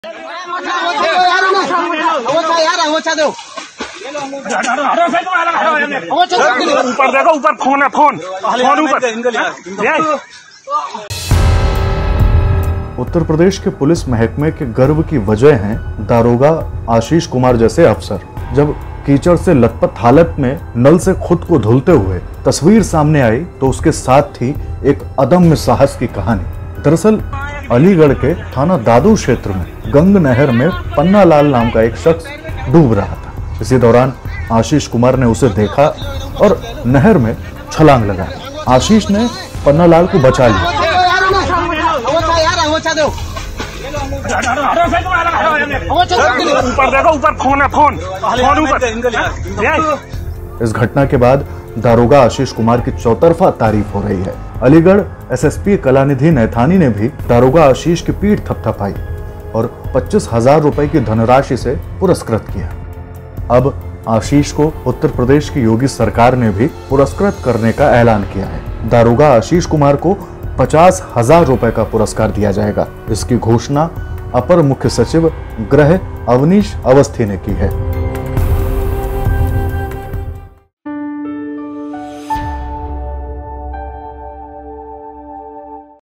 UP Police Good Work: मौत से शख्स को बचा लाया यूपी पुलिस का 'सिंघम', अब इनामों की बारिश! NBT UP-UK
उत्तर प्रदेश के पुलिस महकमे के गर्व की वजह हैं दारोगा आशीष कुमार जैसे अफसर...जब कीचड़ से लथपथ हालत में नल से खुद को धुलते हुए तस्वीर सामने आई तो उसके साथ थी एक अदम्य साहस की कहानी...दरअसल, अलीगढ़ के थाना दादों क्षेत्र में गंगनहर में पन्नालाल नाम का शख्स डूब रहा था। इसी दौरान आशीष कुमार ने उसे देखा और नहर में छलांग लगा दी। आशीष ने पन्नालाल को बचा लिया। इस घटना के बाद दारोगा आशीष कुमार की चौतरफा तारीफ हो रही है। अलीगढ़ एसएसपी कलानिधि नैथानी ने भी दारोगा आशीष की पीठ थपथपाई और 25 हजार रुपये की धनराशि से पुरस्कृत किया। अब आशीष को उत्तर प्रदेश की योगी सरकार ने भी पुरस्कृत करने का ऐलान किया है। दारोगा आशीष कुमार को 50 हजार रुपये का पुरस्कार दिया जाएगा। इसकी घोषणा अपर मुख्य सचिव गृह अवनीश अवस्थी ने की है।